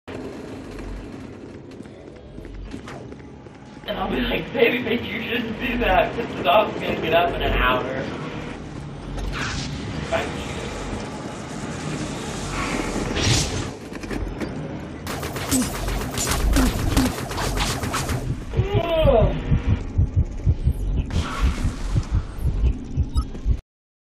[Bug] Scout Speed Slowdown
Heard about this a lot in the Tech Support forums but never had any video of it. Welp, finally got it. Talk about an instant change in speed. Might as well have been walking.